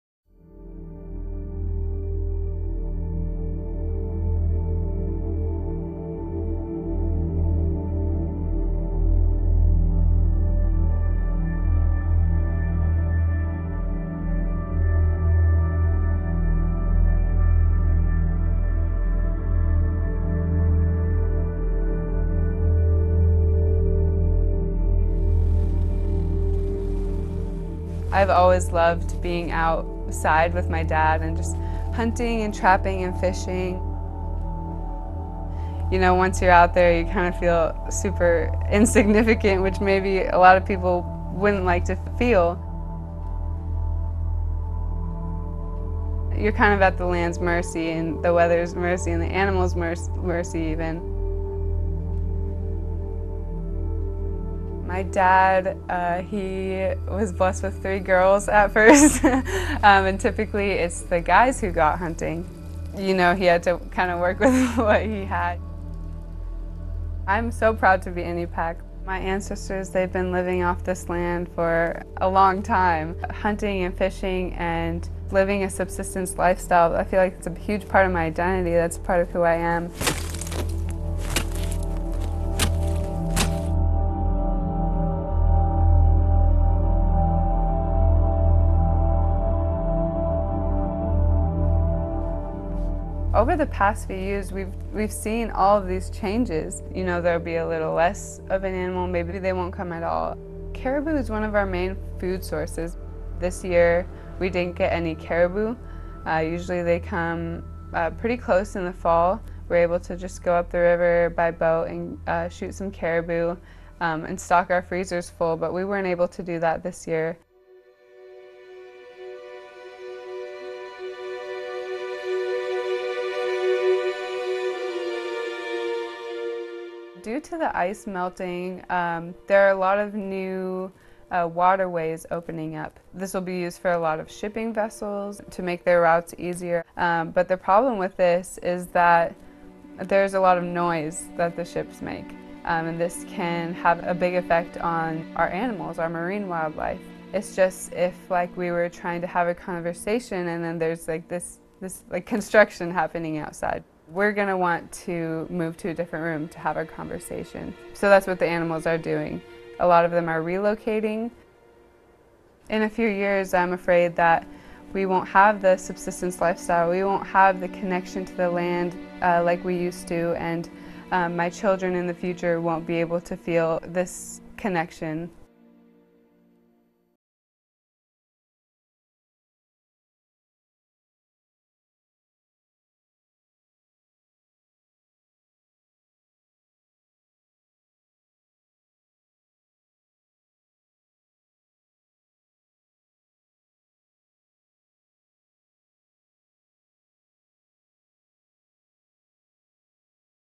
Climate change: The woman watching ice melt from under her feet - BBC World Service, BBC 100 Women
Cassidy Kramer is part of the Inupiaq community, who have relied on Alaska's land for thousands of years. Like her father, she goes hunting and fishing to provide for the people around her. But Cassidy fears that her way of life could be gone before she starts a family of her own.

"In a few years, I'm afraid we won't have the subsistence lifestyle," she says. "We won't have the connection to the land that we used to and my children in the future won't be able to feel this connection."

Around her, the landscape is changing as temperatures warm and the ice melts. Now it's up to her, to hold on to her traditions as best she can.

BBC 100 Women names 100 influential and inspirational women each year and shares their stories.